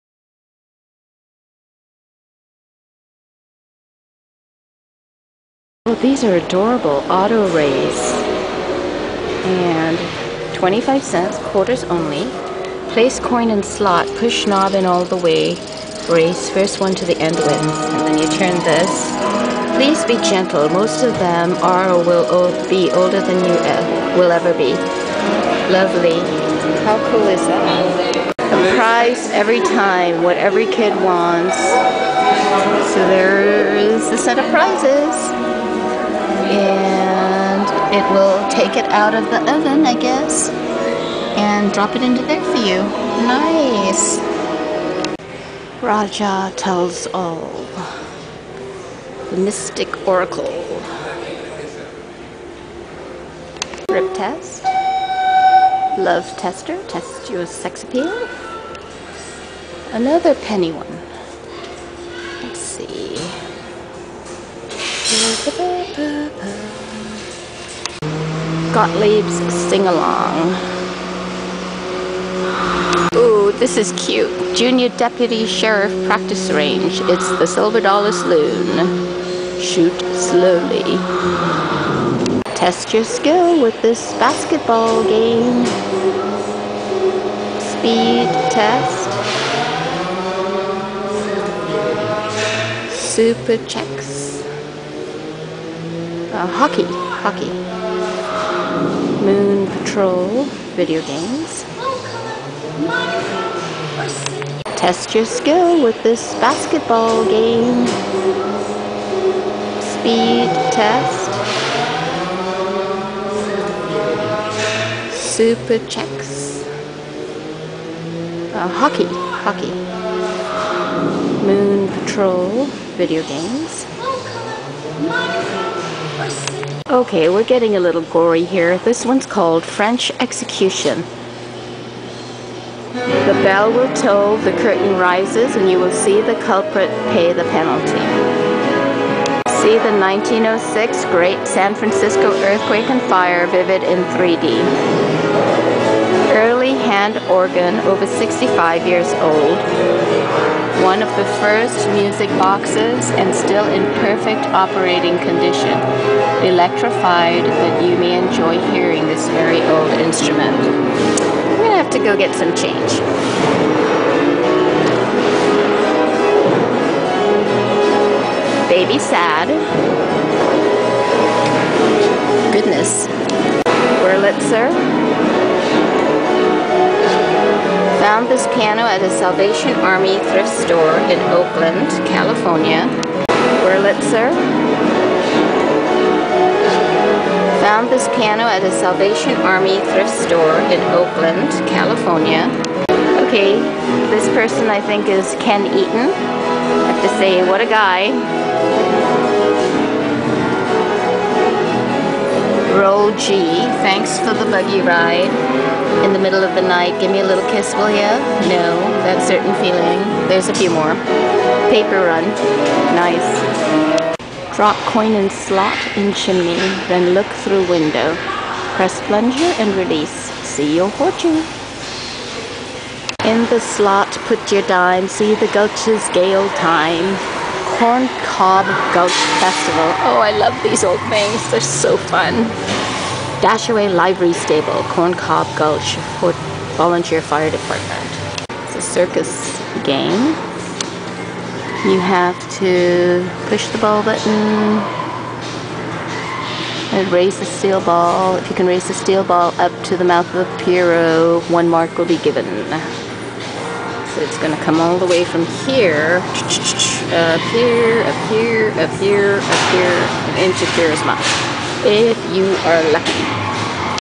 Old mechanical games and pinball machines at San Francisco's Pier 45 (video 1)
These apparently came from the old Cliff House basement (restaurant off of PCH) before the Cliff House was rebuilt.  The Musee Mechanique is free, but you'll want to take some change... Games are only a quarter, most of them, and they're very cool.